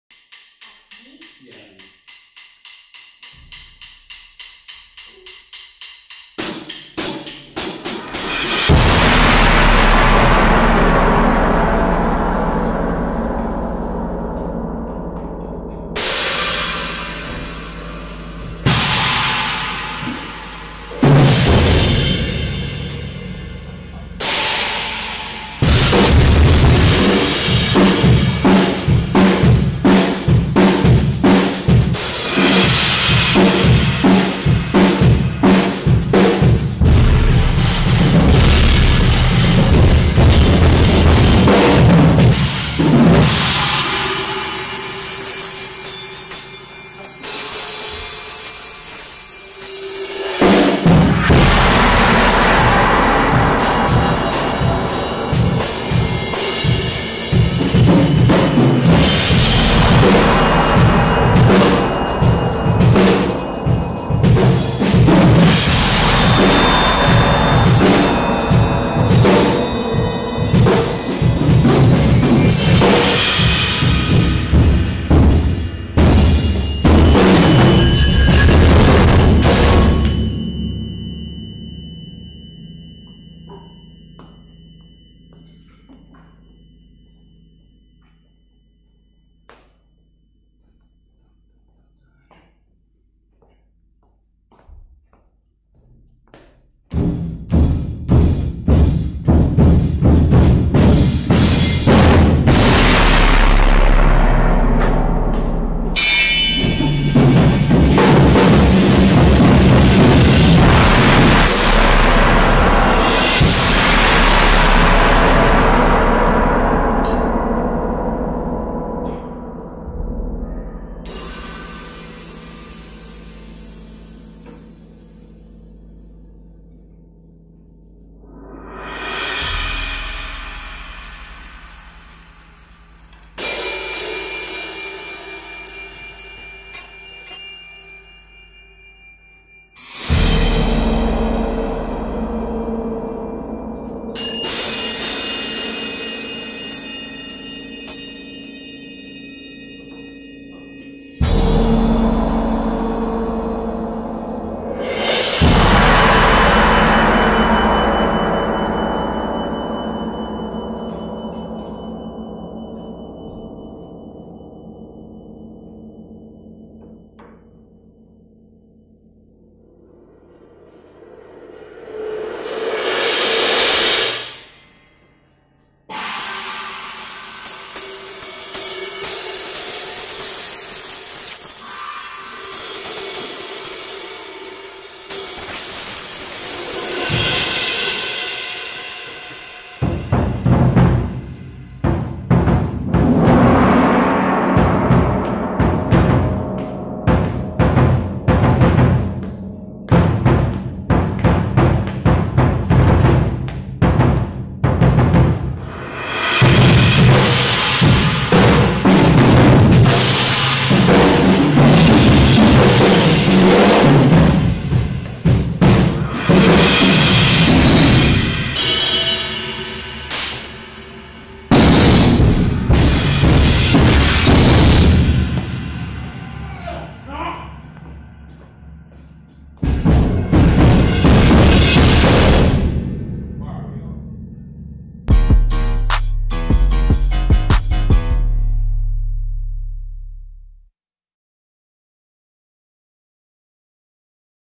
Music City Mystique Racks 2019 | 02.03.19
//\// Headphone Users Beware lolol
//\// Music City Mystique Racks Side One 2019
//\// I decided to upload all the rehearsal runs I've recorded every rehearsal for the entire season haha. Enjoy